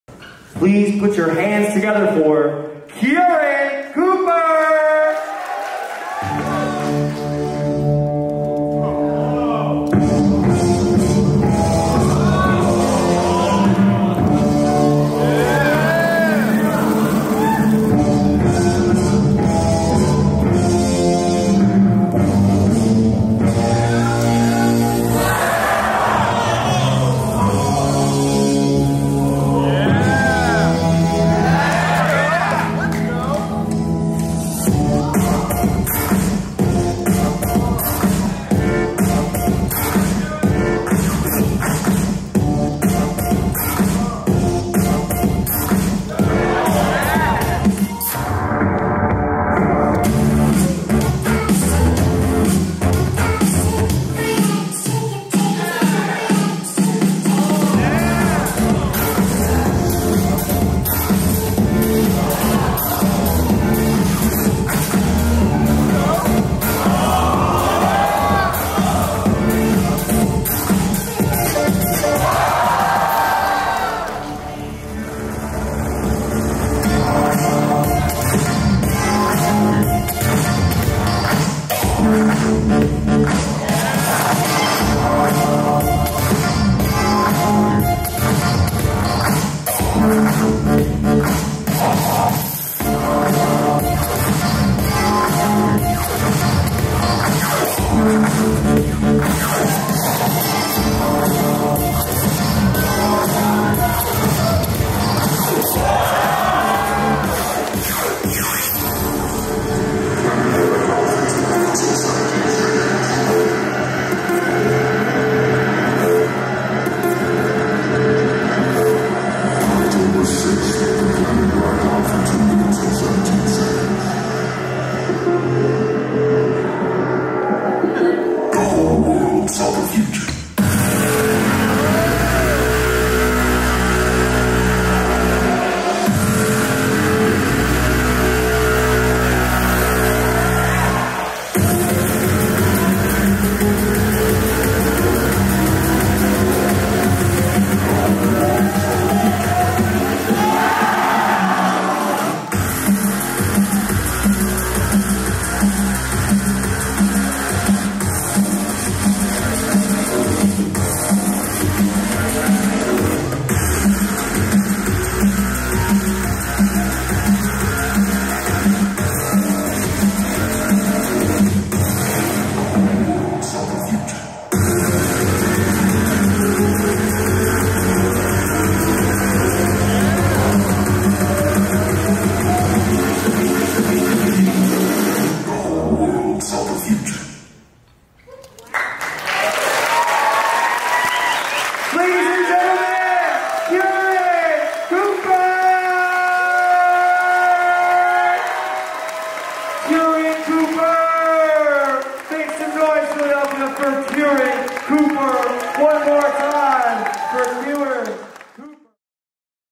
Keiran Cooper - 8th Place - 1A Final - 2019 US Nationals
Keiran Cooper takes 8th place in the 1A division at the National YoYo Contest.

The 2019 US National Yo-Yo Championship was held on Sunday, July 7th at the Independence Seaport Museum in Philadelphia, PA.